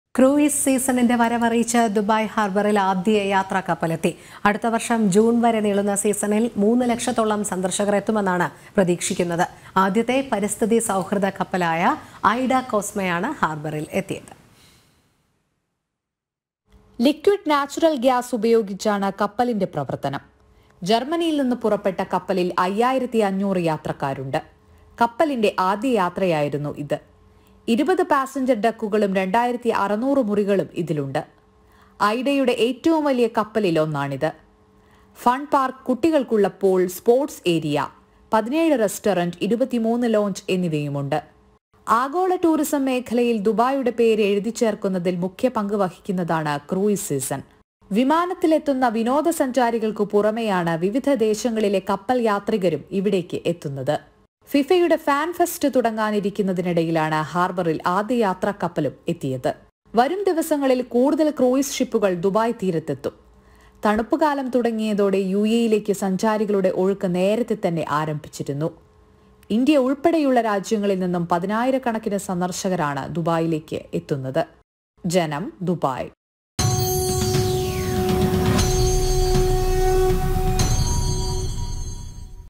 ക്രൂയിസ് സീസണിന്‍റെ വരവറിയിച്ച് ദുബായ് ഹാർബറിൽ ആദ്യ യാത്രക്കപ്പൽ എത്തി | JANAM TV
NEWS ANCHOR : ROHINI MAHESH, ARYA SREE